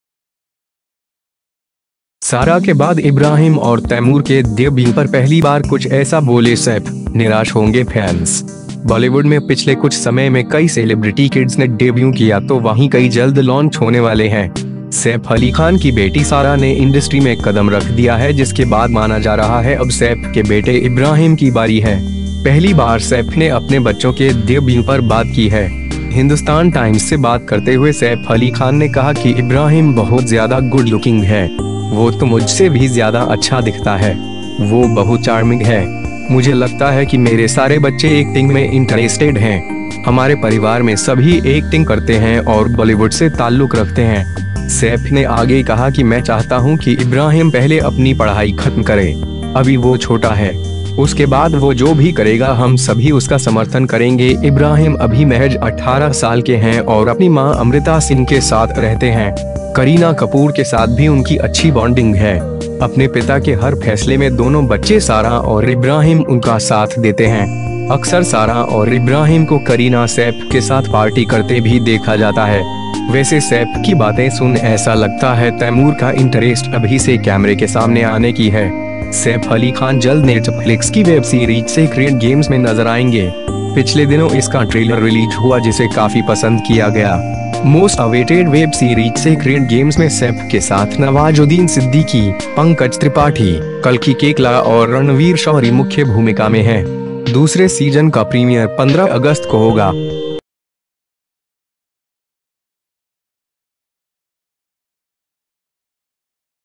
सारा के बाद इब्राहिम और तैमूर के डेब्यू पर पहली बार कुछ ऐसा बोले सैफ, निराश होंगे फैंस
बॉलीवुड में पिछले कुछ समय में कई सेलिब्रिटी किड्स ने डेब्यू किया तो वहीं कई जल्द लॉन्च होने वाले हैं। सैफ अली खान की बेटी सारा ने इंडस्ट्री में कदम रख दिया है जिसके बाद माना जा रहा है अब सैफ के बेटे इब्राहिम की बारी है। पहली बार सैफ ने अपने बच्चों के डेब्यू पर बात की है।

हिंदुस्तान टाइम्स से बात करते हुए सैफ अली खान ने कहा कि 'इब्राहिम बहुत ज्यादा गुड लुकिंग है। वो तो मुझसे भी ज्यादा अच्छा दिखता है। वो बहुत चार्मिंग है। मुझे लगता है कि मेरे सारे बच्चे एक्टिंग में इंटरेस्टेड हैं। हमारे परिवार में सभी एक्टिंग करते हैं और बॉलीवुड से ताल्लुक रखते हैं।'

सैफ ने आगे कहा कि 'मैं चाहता हूं कि इब्राहिम पहले अपनी पढ़ाई खत्म करे। अभी वो छोटा है। उसके बाद वो जो भी करेगा हम सभी उसका समर्थन करेंगे।' इब्राहिम अभी महज 18 साल के हैं और अपनी मां अमृता सिंह के साथ रहते हैं। करीना कपूर के साथ भी उनकी अच्छी बॉन्डिंग है। अपने पिता के हर फैसले में दोनों बच्चे सारा और इब्राहिम उनका साथ देते हैं। अक्सर सारा और इब्राहिम को करीना-सैफ के साथ पार्टी करते भी देखा जाता है। वैसे सैफ की बातें सुन ऐसा लगता है तैमूर का इंटरेस्ट अभी से कैमरे के सामने आने की है।

सैफ अली खान जल्द नेटफ्लिक्स की वेबसीरीज सेक्रेड गेम्स में नजर आएंगे। पिछले दिनों इसका ट्रेलर रिलीज हुआ जिसे काफी पसंद किया गया। मोस्ट अवेटेड वेबसीरीज सेक्रेड गेम्स में सैफ के साथ नवाजुद्दीन सिद्दीकी, पंकज त्रिपाठी, कल्कि केकला और रणवीर शौरी मुख्य भूमिका में हैं। दूसरे सीजन का प्रीमियर 15 अगस्त को होगा।